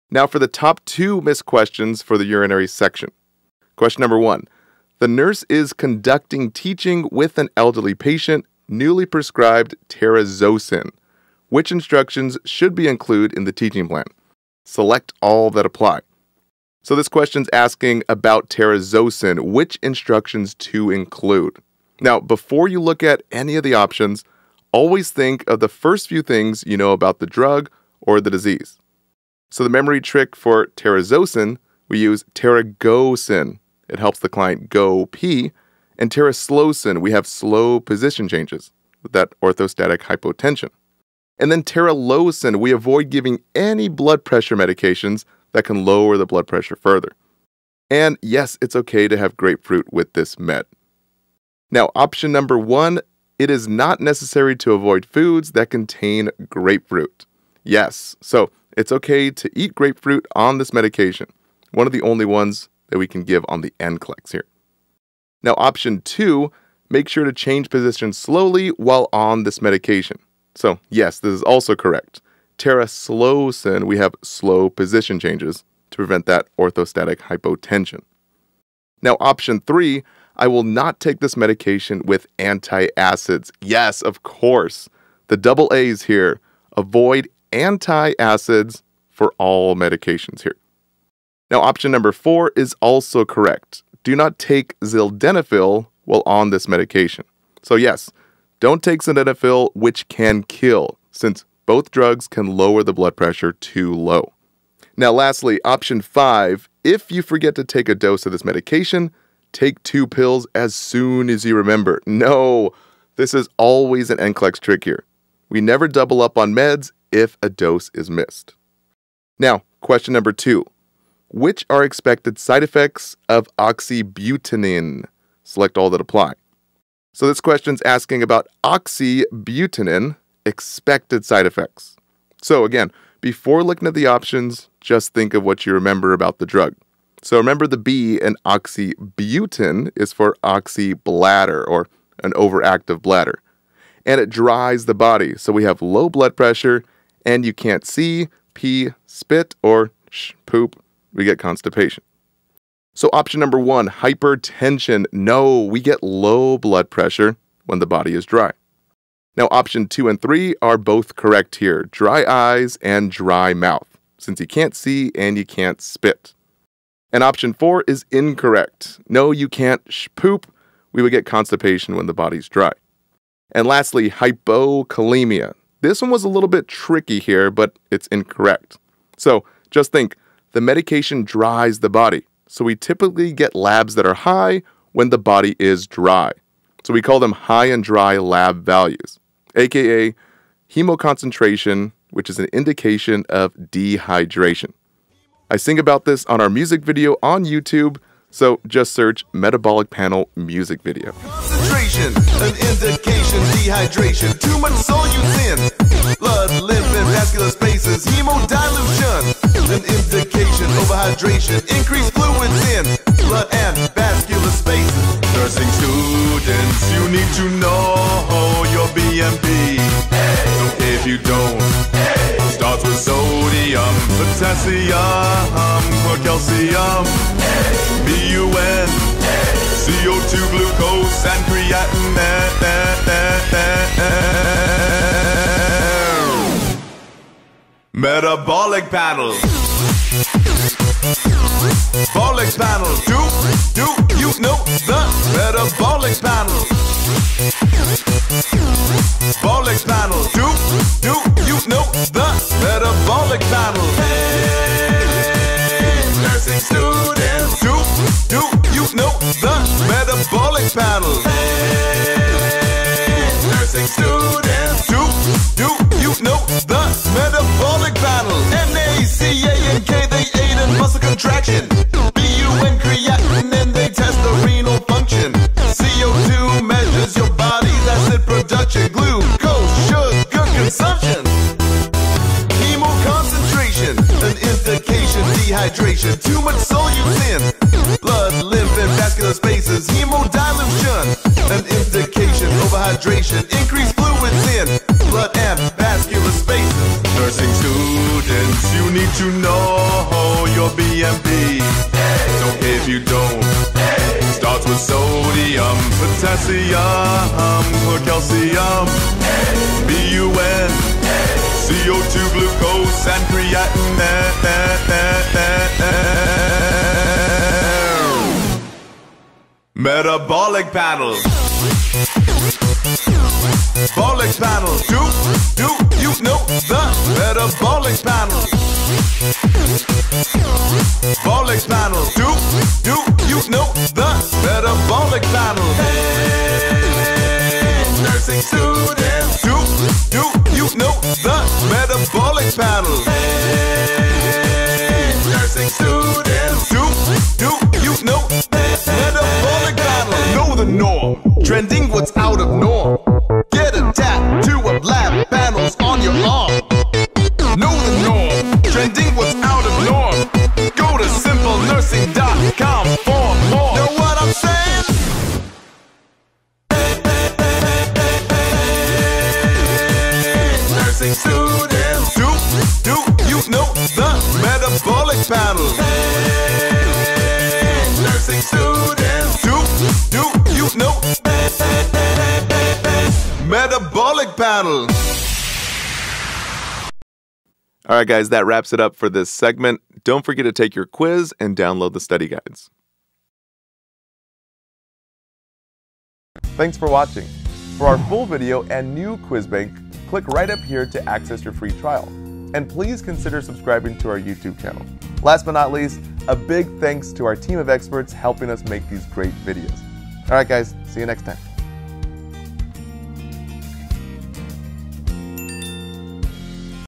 Pharmacology - BPH (Benign Prostatic Hyperplasia) NCLEX top 2 missed questions RN PN NCLEX
Today’s video is all about benign prostatic hyperplasia (BPH) for Nursing Students and NCLEX Review.

Benign prostatic hyperplasia (BPH) is a condition that affects the prostate gland, and is typically related to aging. It's characterized by an increased size of the prostate and symptoms like difficulty urinating, frequency of urination, and decreased urine flow.

Watch to learn more about BPH pathophysiology, nursing interventions, and more.